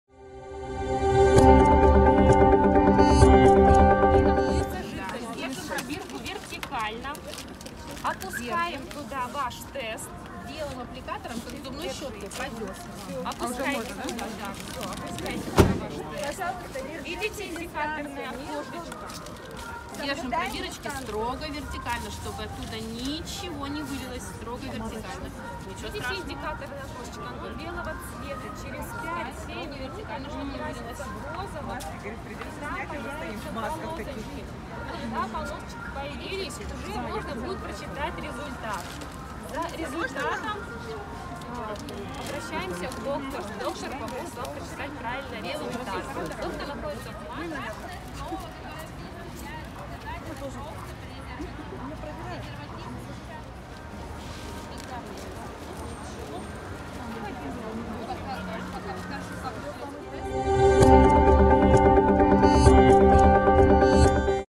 Хабаровчане сдавали тест на ВИЧ-инфекцию по пути в торговый центр
Десятки хабаровчан смогли анонимно и бесплатно узнать свой ВИЧ-статус по пути в торговый центр. Это стало возможным благодаря акции Минздрава «Тест на ВИЧ: Экспедиция 2020».  Брендированная "Газель" едет через всю Россию и делает остановки в городах. Подробнее  читайте в материале на DVHab.ru